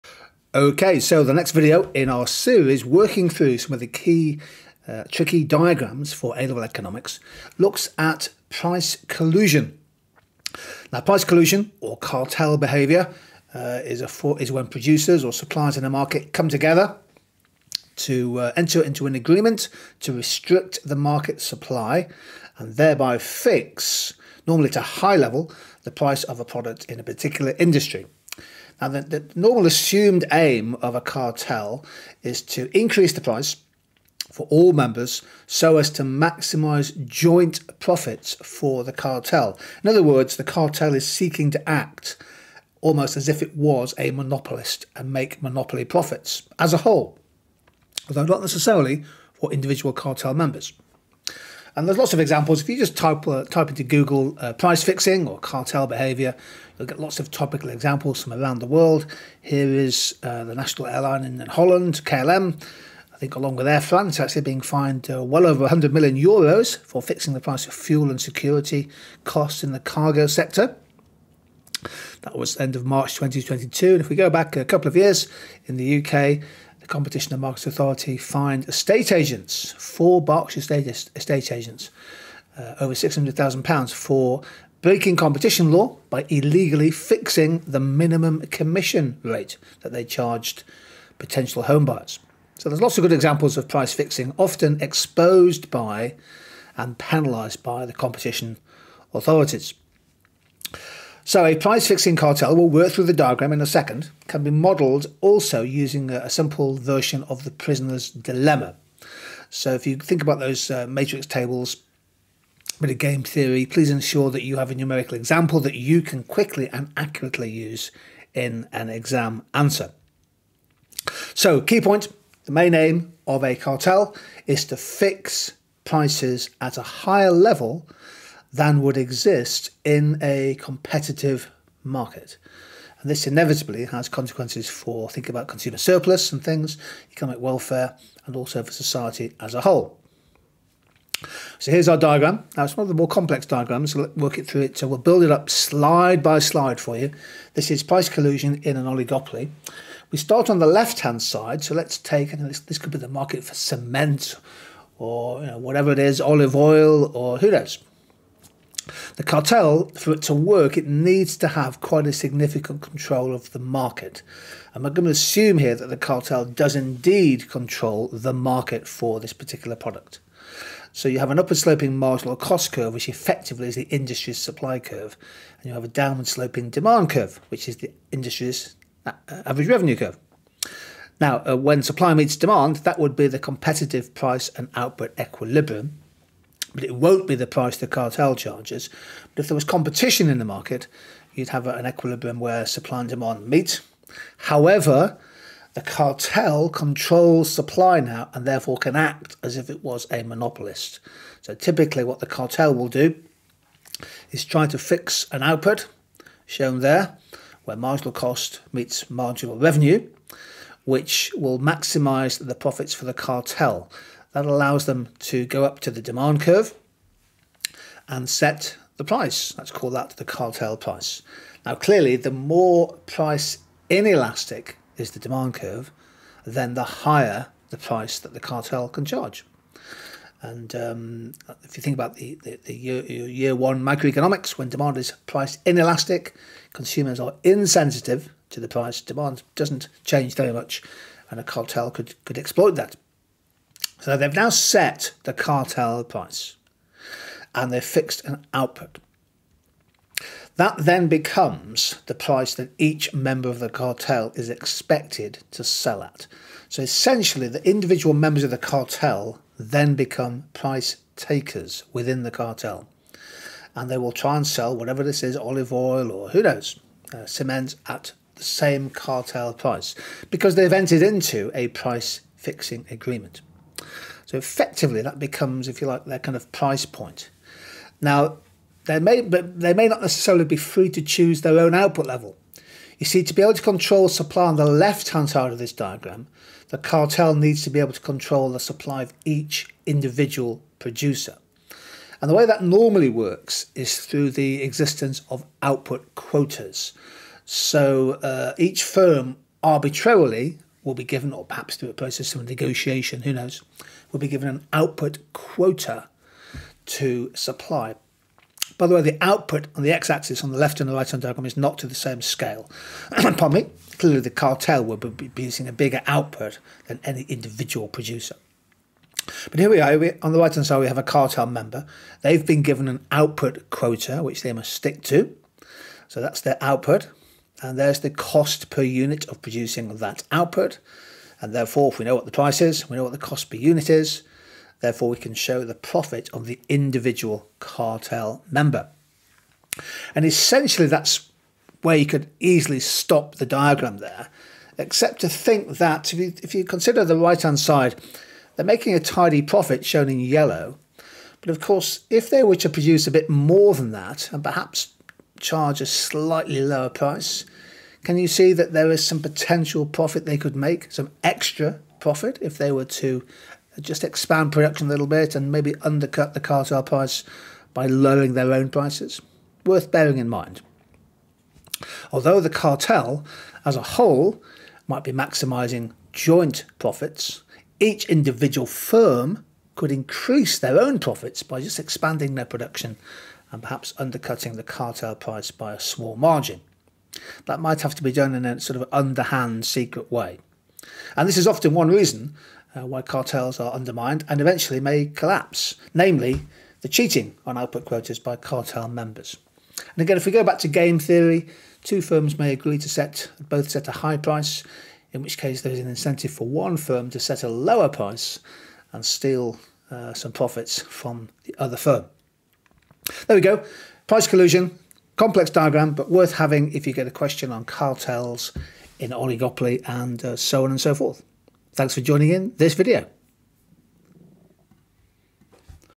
Key Diagrams - Price Collusion in a Cartel (Oligopoly)
In this video we work through a price-fixing diagram associated with cartel behaviour in an oligopoly.